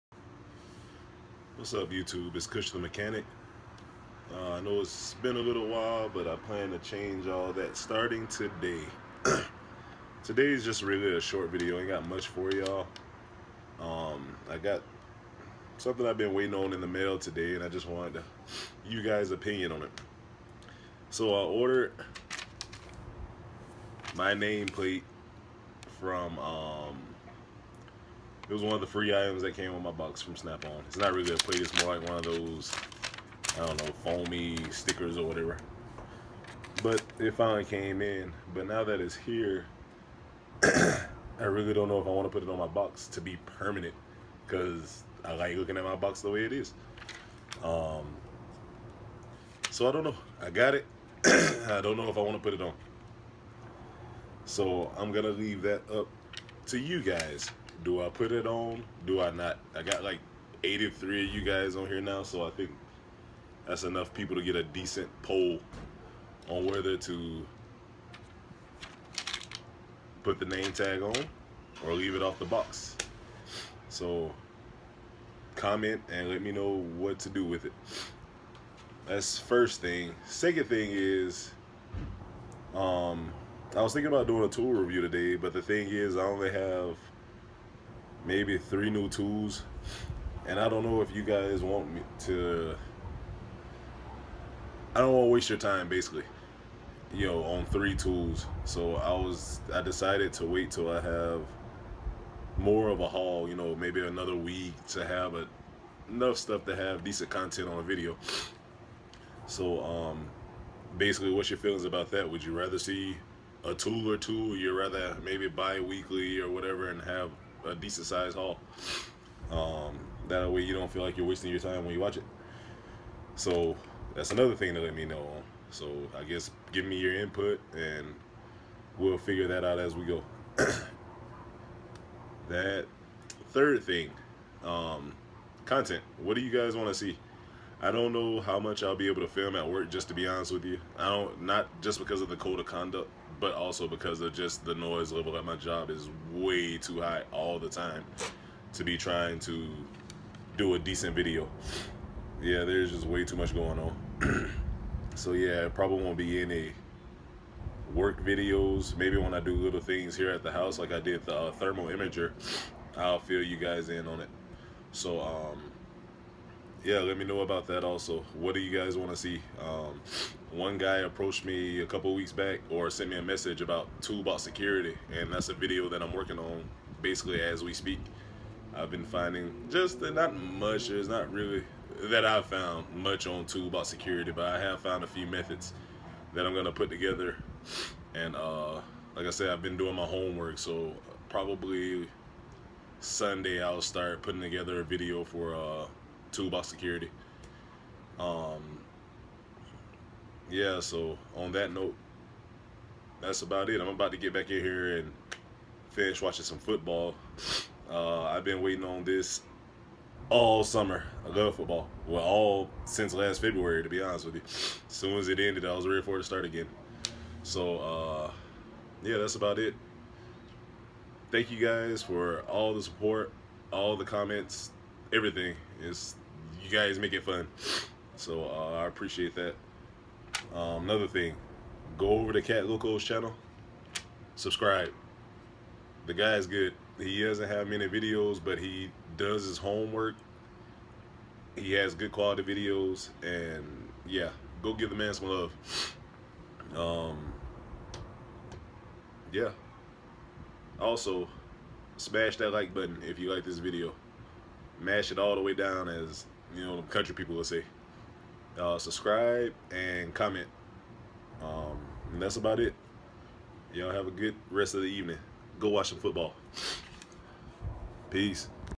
WHAT DO I DO???
Name plate, to put it on or not to put it on, that is the question.